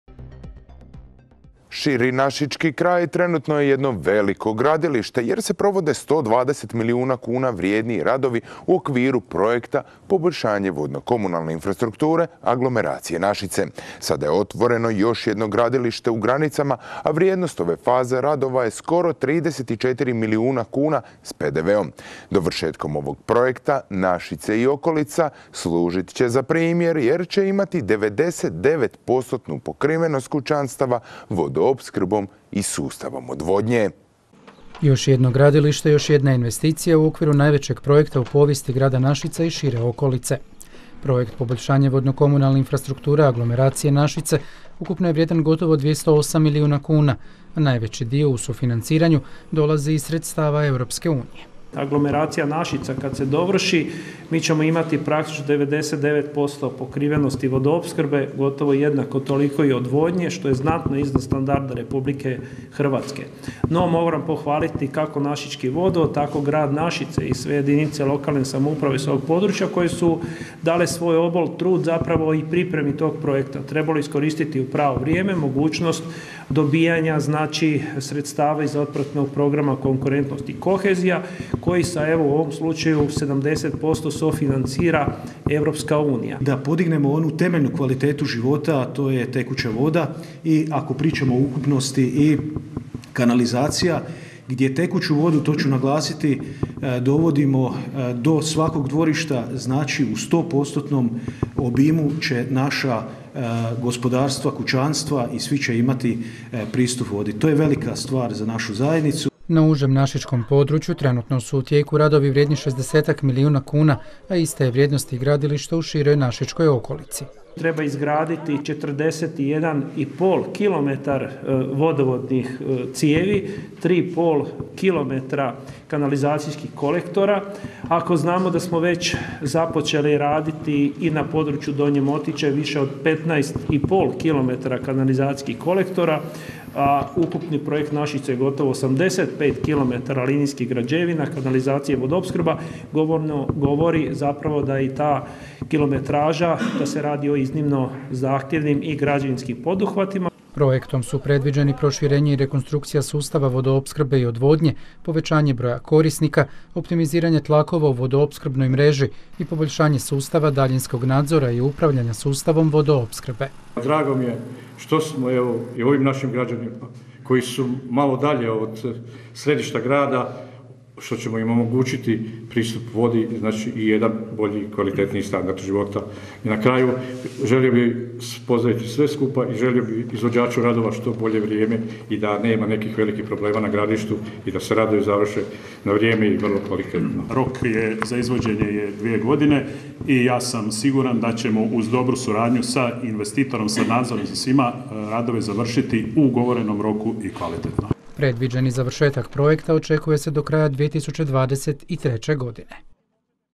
Radovi u sklopu Aglomeracije Našice započeli i u Granicama
U Gljivarskom domu u Granicama održana je tiskovna konferencija EU projekta "Poboljšanje vodnokomunalne infrastrukture aglomeracije Našice", povodom početka izvođenja radova na izgradnji linijskih objekata šireg našičkog područja - Aktivnost 4: Dogradnja vodnokomunalne infrastrukture šireg našičkog područja. Vrijednost radova Aktivnost 4: Dogradnja vodnokomunalne infrastrukture šireg našičkog područja iznosi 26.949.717,54 kn bez PDV-a, odnosno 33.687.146,93 kn s PDV-om.

Konferenciji su prisustvovali Zoran Đuroković, generalni direktor Hrvatskih voda, Davor Vukmirić, zamjenik generalnog direktora Hrvatskih voda, Željko Kovačević, direktor VGO Osijek, Krešimir Kašuba, gradonačelnik grada Našice, Oto Dudjak, predsjednik uprave Našičkog vodovoda d.o.o., Zoran Škorić, predstavnik Zajednice izvođača – OSIJEK KOTEKS d.d. Osijek, Vodovod - montaža d.o.o. Osijek i Gravia d.o.o. Osijek i drugi uvaženi gosti.

Konferencijom u Granicama službeno je otvoreno i zadnje gradilište u sklopu projekta Aglomeracije Našice što je dokaz da se brine o svakom, pa i najmanjem kutku Hrvatske izjavio je Zoran Đuroković, generalni direktor Hrvatskih voda.